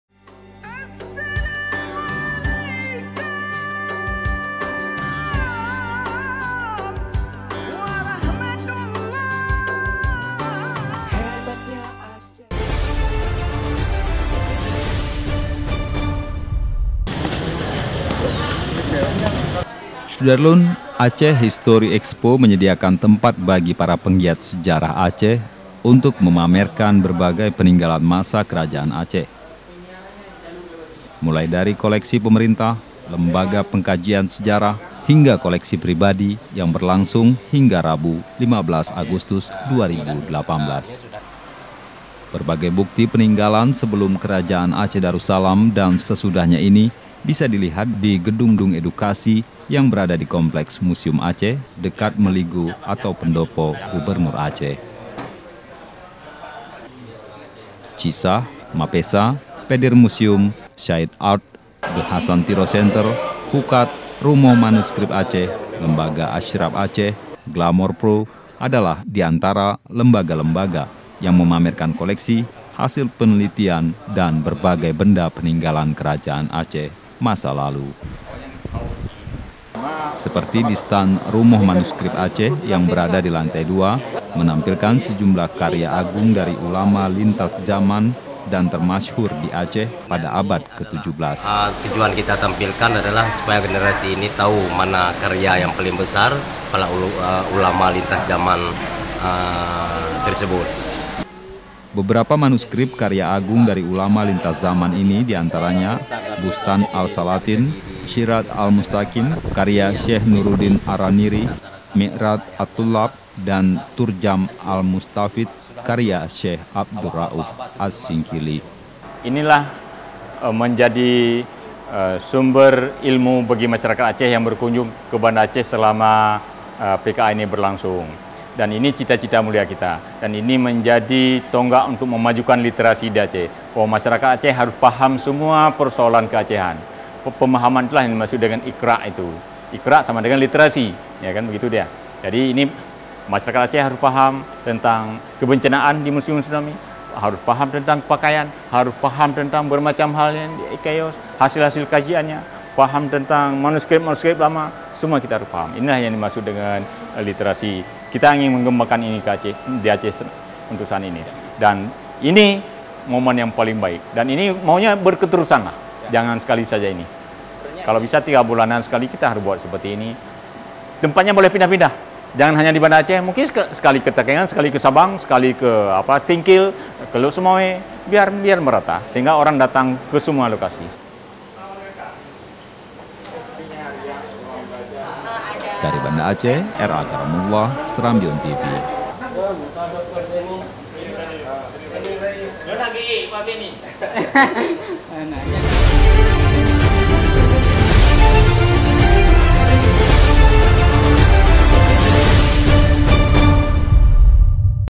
Koleksi Pribadi Sejarawan Aceh Banyak Dipamerkan di Aceh History Expo
SERAMBITV.CO, BANDA ACEH - Aceh History Expo menyediakan tempat bagi para pegiat sejarah Aceh untuk memamerkan berbagai peninggalan masa Kerajaan Aceh, mulai dari koleksi pemerintah, lembaga pengkajian sejarah, hingga koleksi pribadi yang berlangsung hingga Rabu (15/8/2018).

Berbagai bukti peninggalan sebelum Kerajaan Aceh Darussalam dan sesudahnya ini bisa dilihat di gedung gedung edukasi yang berada di Kompleks Museum Aceh, dekat Meuligoe (Pendopo) Gubernur Aceh.

Cisah, MAPESA, Pedir Museum, Sayed Art, The Hasan Tiro Centre, Pukat, Rumoh Manuskrip Aceh, Lembaga Asyraf Aceh, Glamour Pro adalah di antara lembaga-lembaga yang memamerkan koleksi, hasil penelitian, dan berbagai benda peninggalan Kerajaan Aceh masa lalu.

Seperti di stand Rumoh Manuskrip Aceh yang berada di lantai dua, menampilkan sejumlah karya agung dari ulama lintas zaman dan termasyhur di Aceh pada abad ke 17.

Beberapa manuskrip karya agung dari ulama lintas zaman ini di antaranya; Bustan Al Salatin, Sirat Al Mustaqim karya Syekh Nuruddin Ar Raniry, Mir'at Ath-Thullab dan Turjaman Al Mustafid karya Syekh Abdurrauf As Singkili.